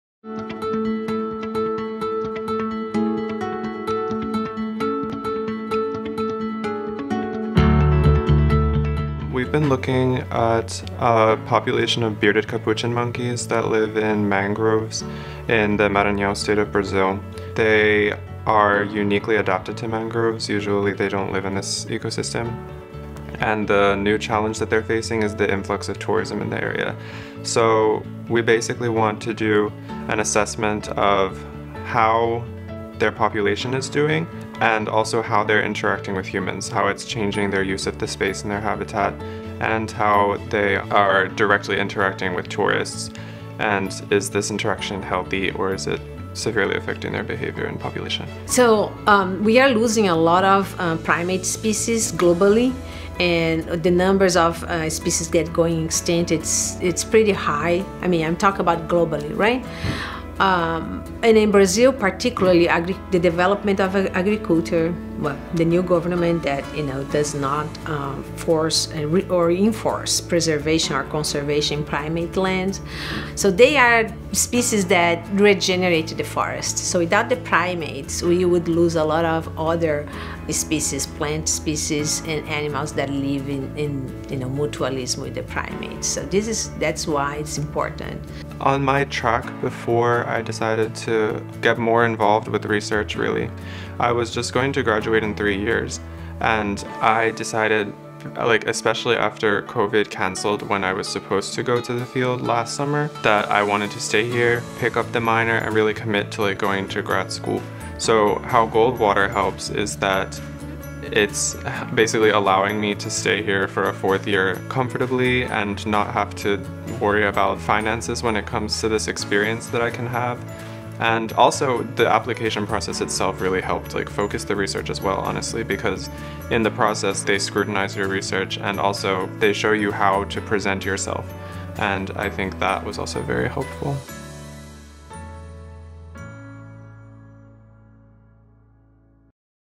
Goldwater Scholarship Helps Salisbury Student Research Capuchin Monkeys in Brazil
Geography major Christian Ciattei is one of three Salisbury University students this year to receive the Barry Goldwater Scholarship, the preeminent undergraduate academic award in the natural sciences, engineering and mathematics. Working with SU’s Dr. Andrea Presotto, Ciattei is researching the interactions of humans and bearded capuchin monkeys in Brazil, with the hopes to improve conservation efforts.